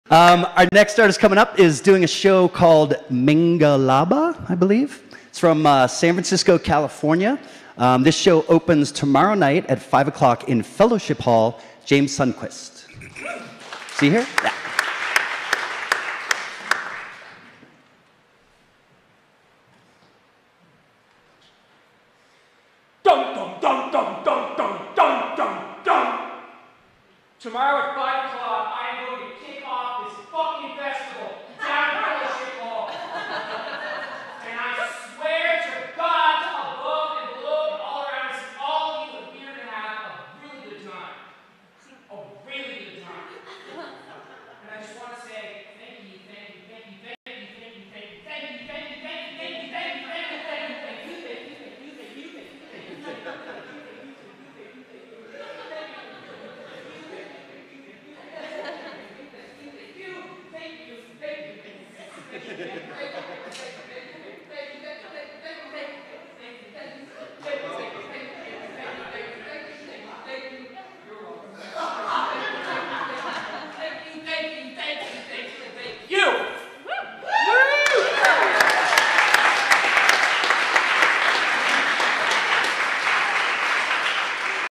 Boulder Fringe MINGALABA preview for James Sundquist
Opening Ceremony Show Preview at Boulder Fringe for MINGALABA by James Sundquist (San Francisco)

Details for the 2019 Boulder Fringe Festival are located at boulderfringe.com